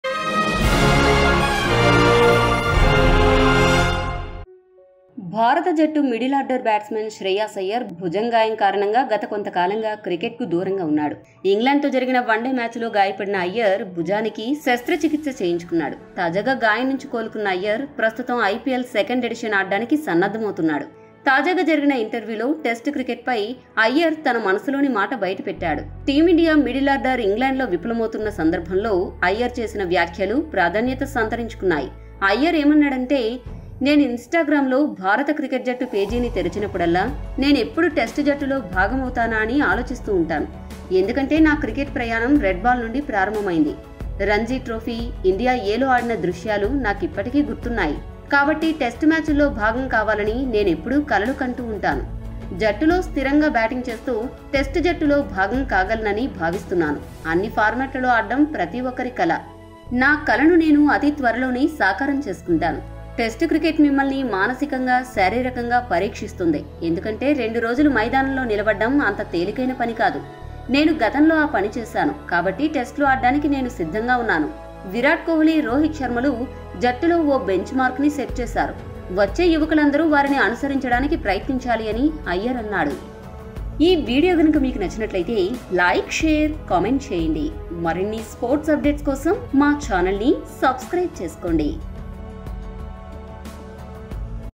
Shreyas Iyer Comments On Playing Test Cricket For Team India|ENG vs IND 4th Test Updates
Shreyas Iyer Comments On Playing Test Cricket For Team India|ENG vs IND 4th Test Updates|Filmy Poster